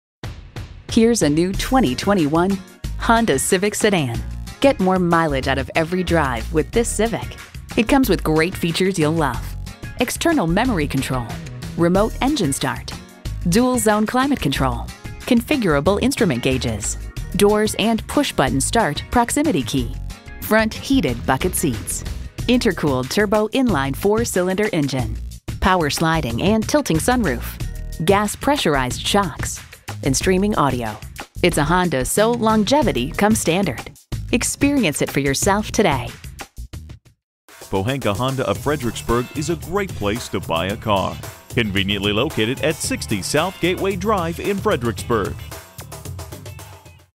New 2021 Honda Civic Fredericksburg VA Richmond, VA FMH703232 - SOLD
Year: 2021
Make: Honda
Model: Civic
Trim: EX
Engine: 1.5L I-4 DI DOHC Turbocharged
Transmission: CVT
Color: Gray
Interior: gray
Stock #: FMH703232
VIN: 2HGFC1F31MH703232

Address:
60 South Gateway Drive